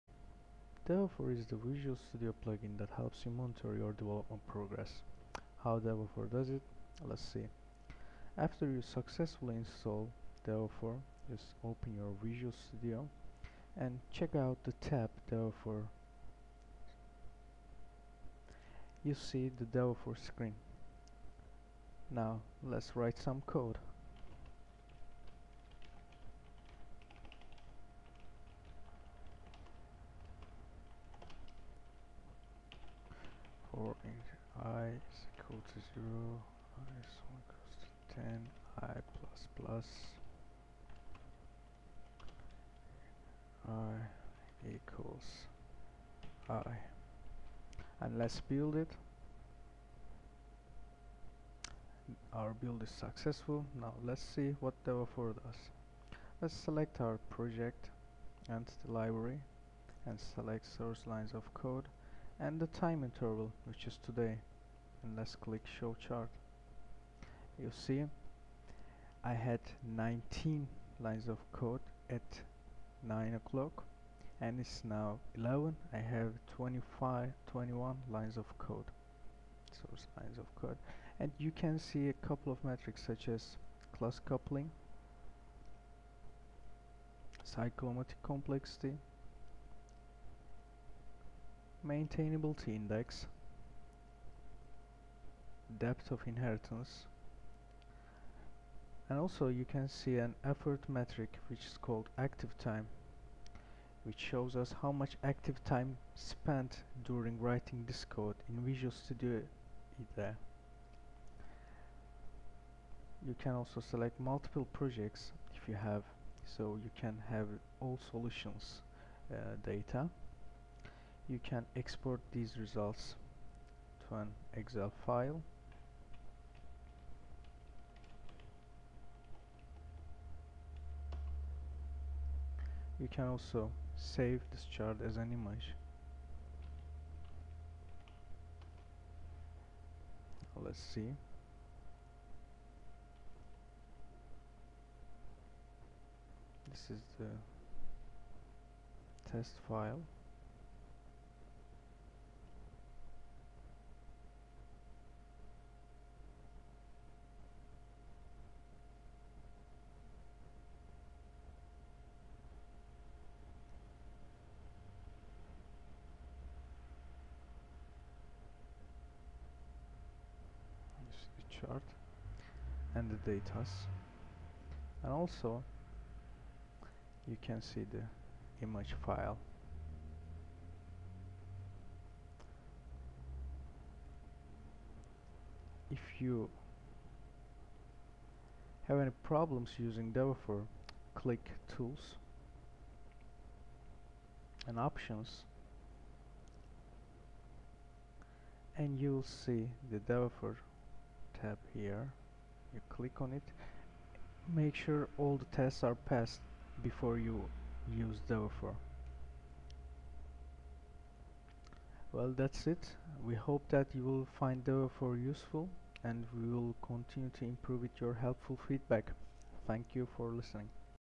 dev \ efor Visual Studio plug-in
dev \ efor is the visual studio plug in that helps you monitor your development progress